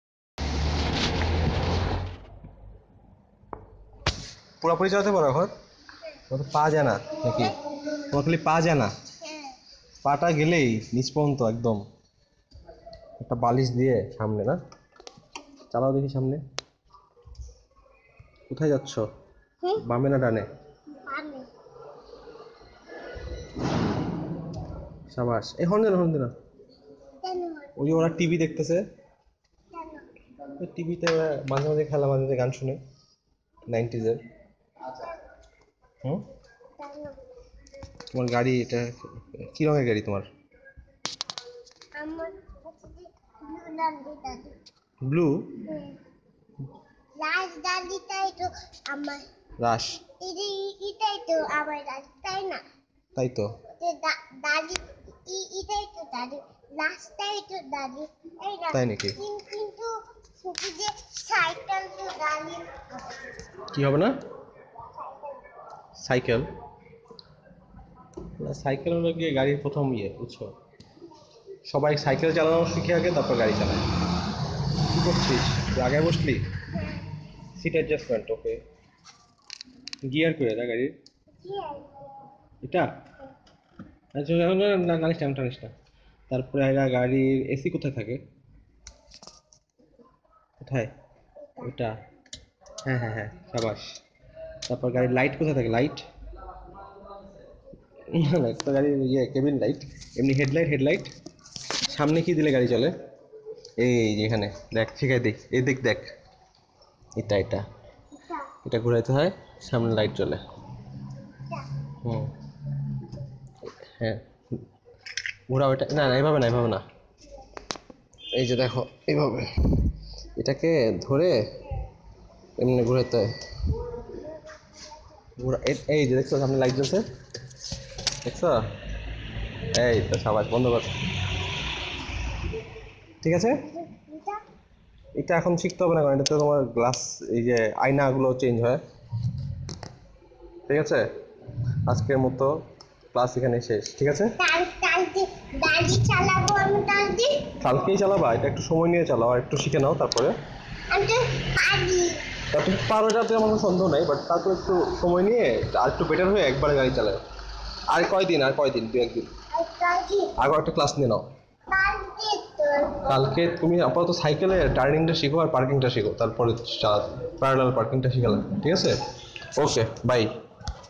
Ayra & her Toyota Rush 🚸 🚫 Young Car ☮ Enthusiast taking the Driver's Seat
at her home parking garage, a winter evening in December 2016, after getting back from her daycare & work !!!
Ms Ayra Mahbub